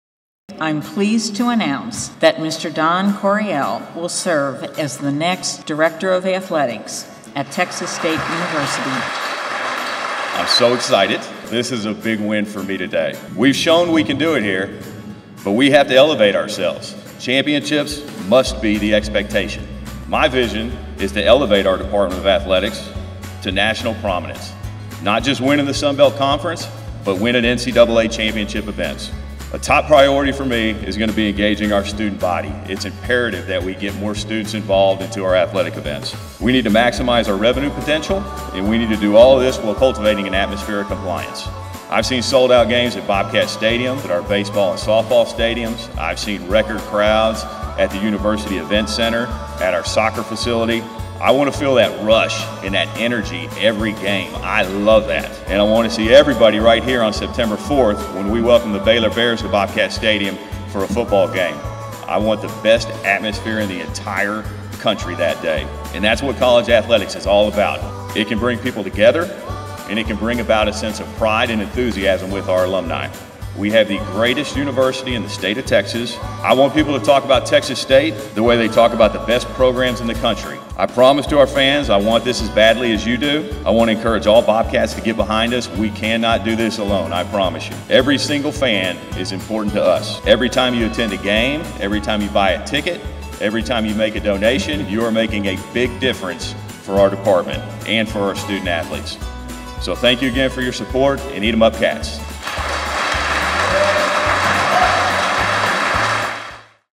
Don Coryell, TXST Director of Athletics
Texas State introduces Don Coryell as the next Director of Athletics.